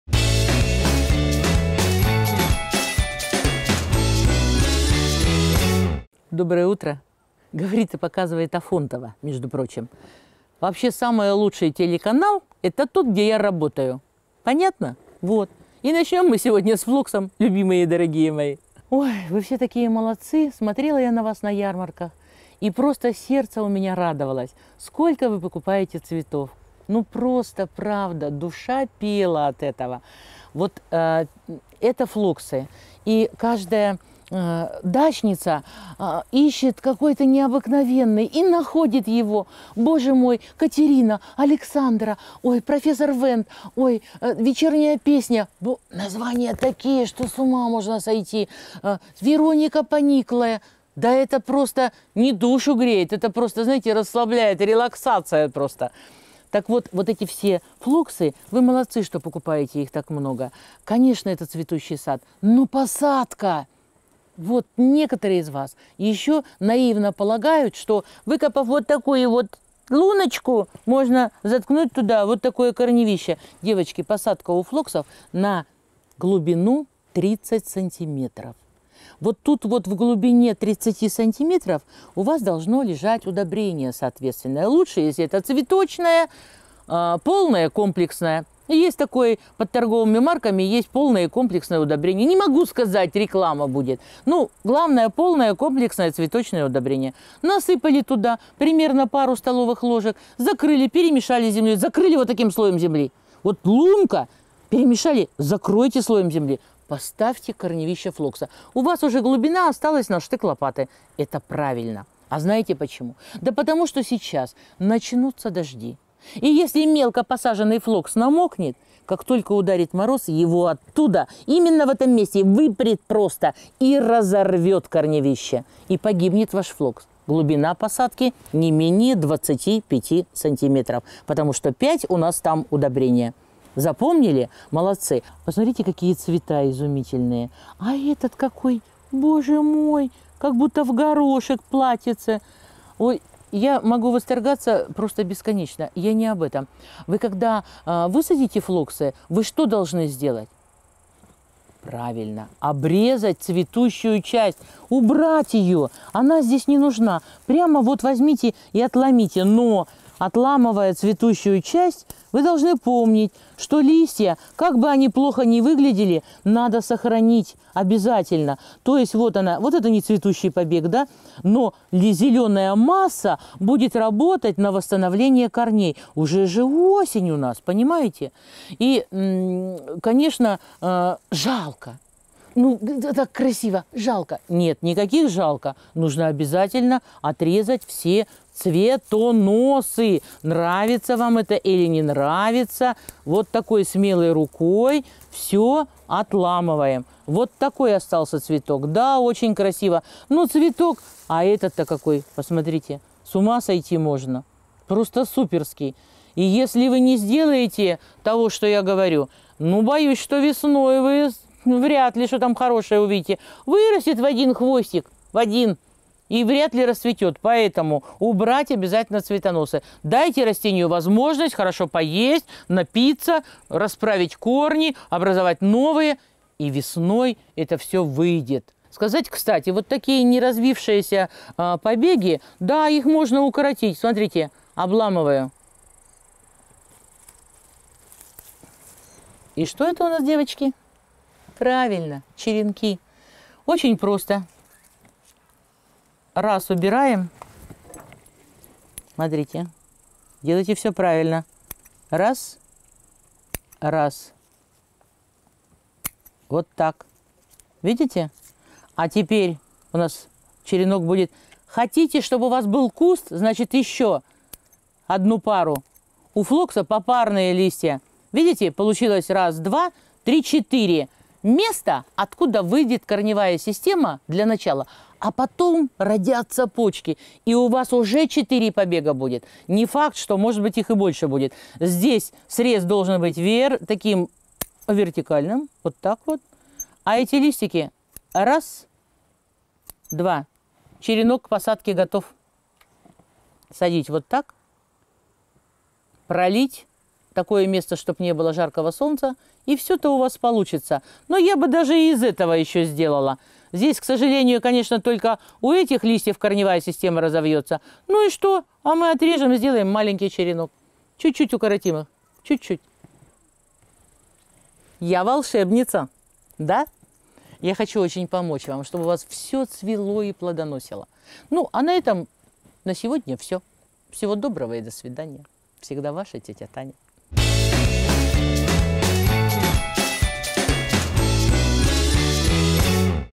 Голова садовая - Как правильно посадить Флоксы
Флоксы - одни из самых замечательных цветов для любого садовода. Цветущий сад это великолепно. Но при неправильной посадке Флоксы не дают всей своей красоты. А я хочу рассказать вам как сделать ваш сад по настоящему цветущим!